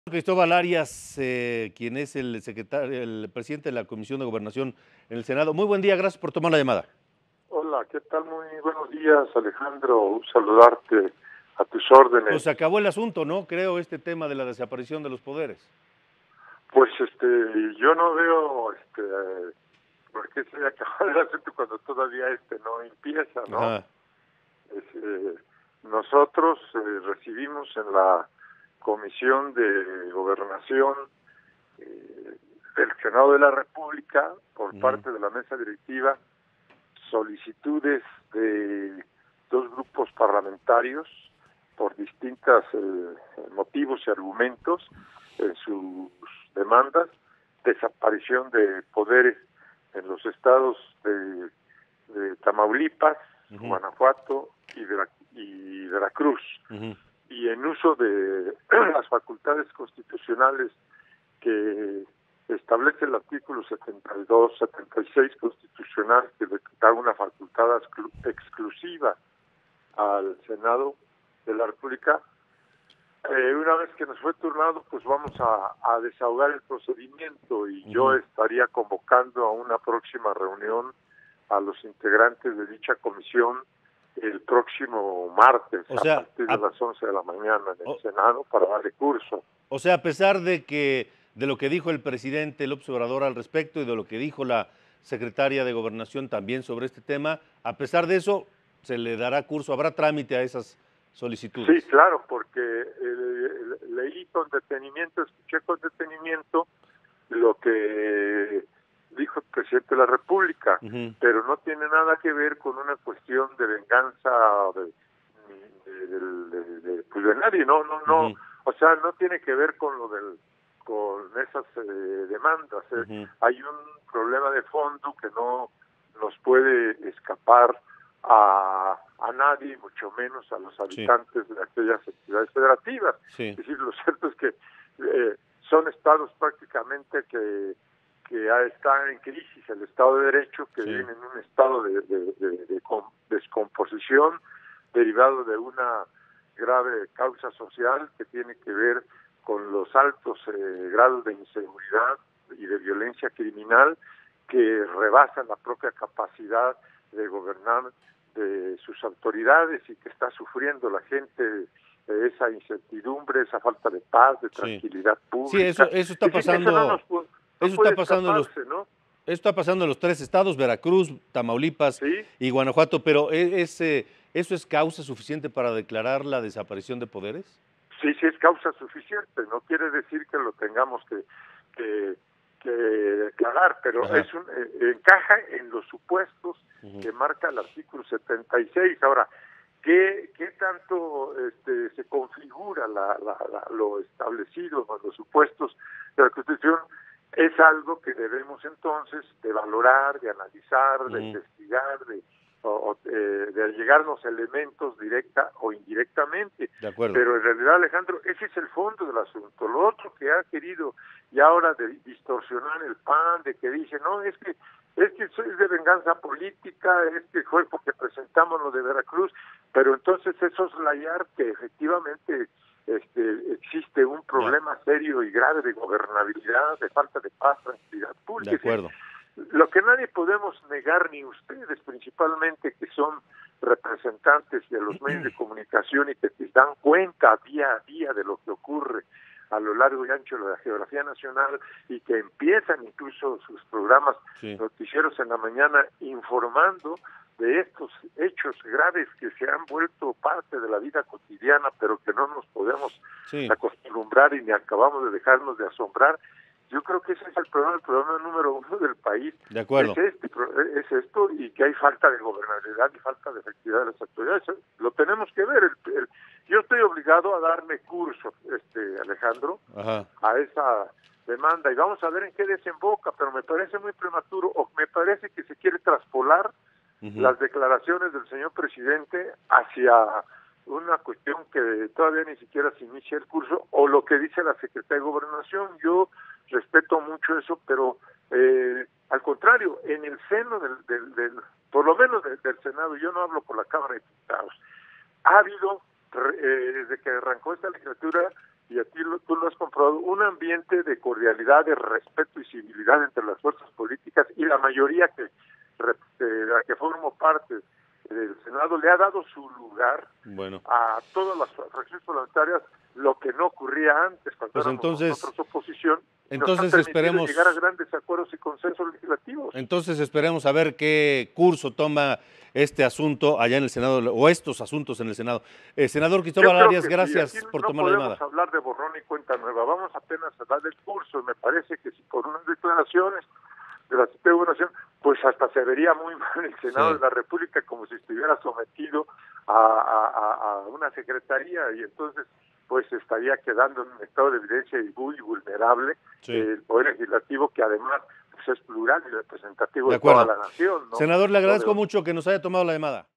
Desaparición de poderes sí se analizará, asegura Cristóbal Arias
Sin importar las palabras de la secretaría de Gobernación, Olga Sánchez Cordero, y el presidente Andrés Manuel López Obrador, se discutirá la pertinencia de que se lleve a cabo la desaparición de poderes en tres estados de la República Mexicana.